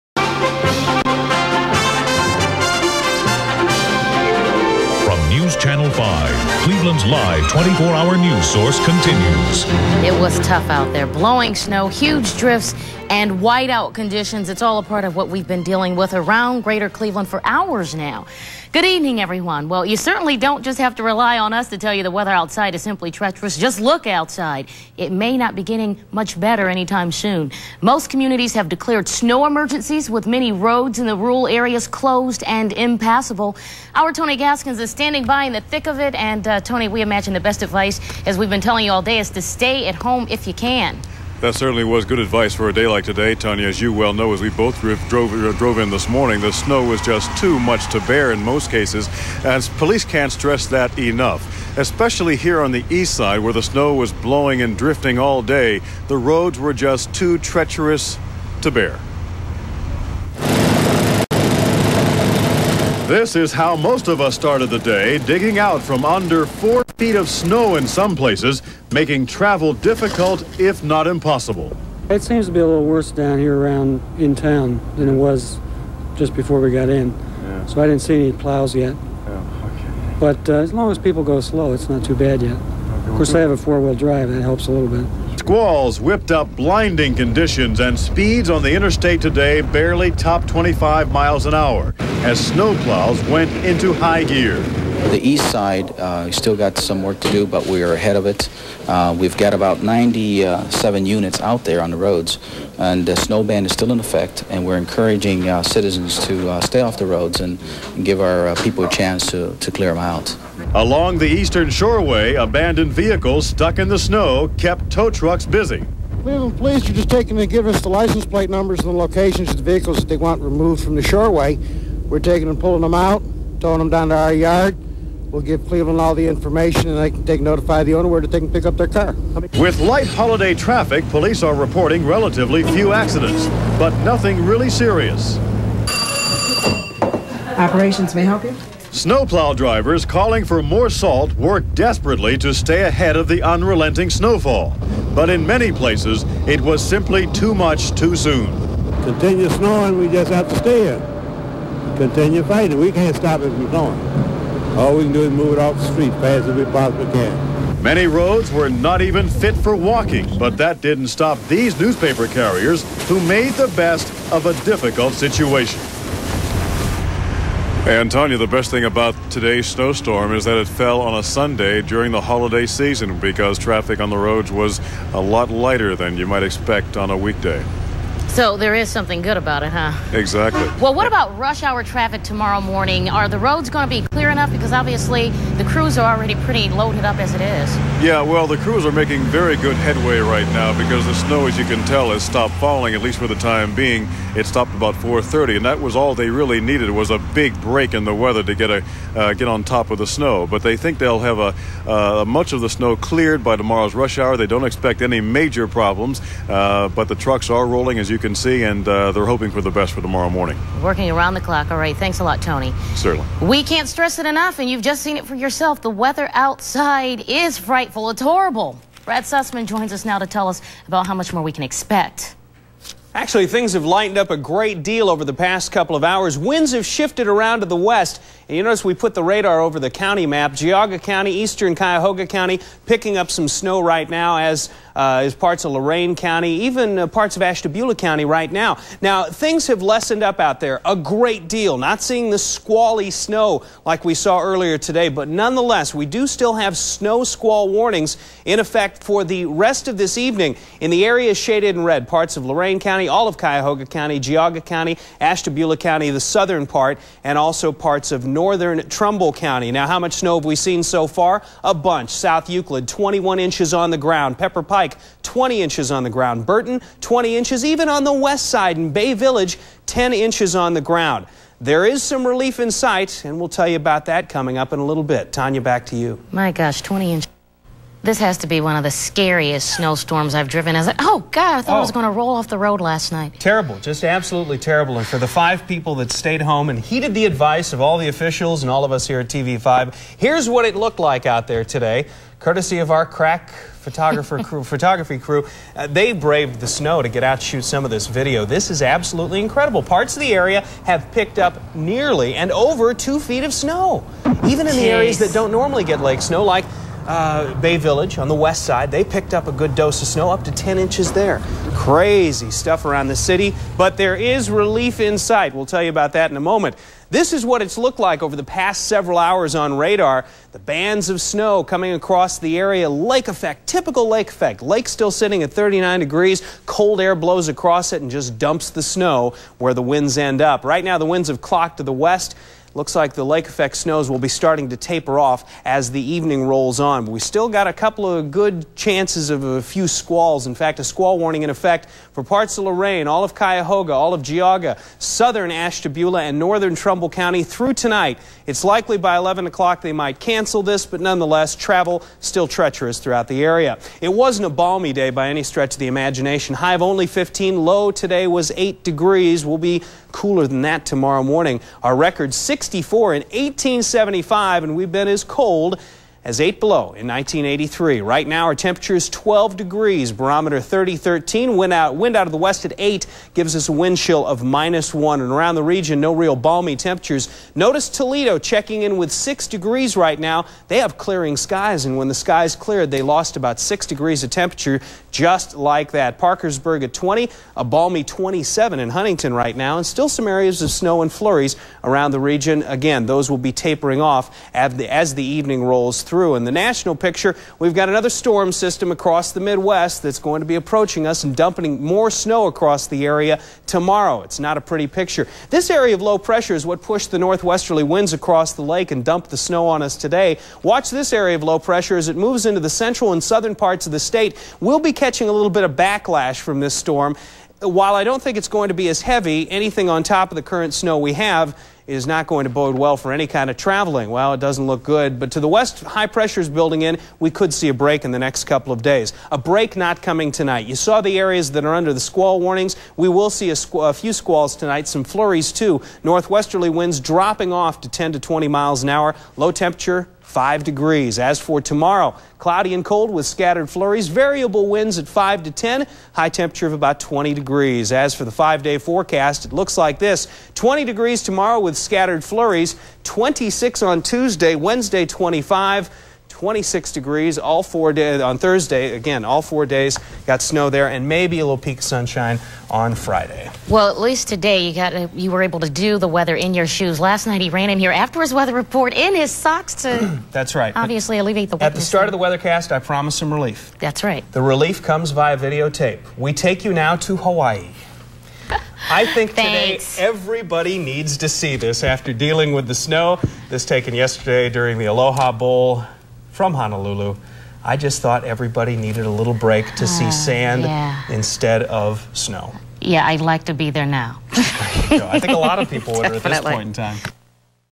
WEWS-TV5 Cleveland - Blizzard News from Dec., 1993!
From Dec. of 1993, here is some more Cleveland blizzard news, from WEWS, Ch. 5.